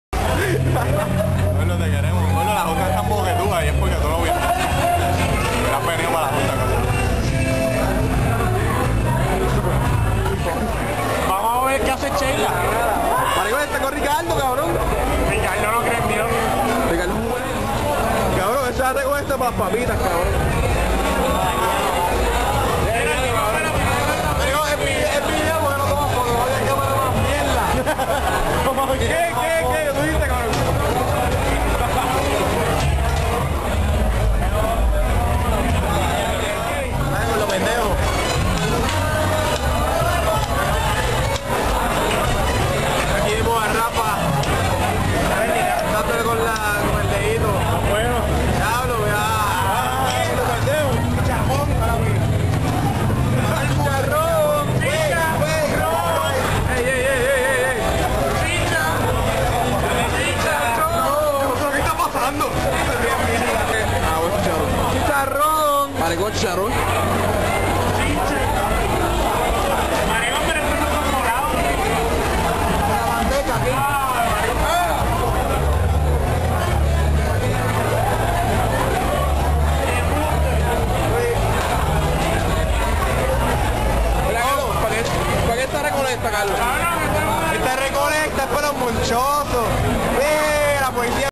aburrido en el garabato en mayaguez
aburrido en el garabato en mayaguez con los panas y si alguien se siente ofendido por el contenido del video no me importa y sime preguntas me cagare en tu madre. edit para decir que lo de aburrimiento es sarcastico no sean brutos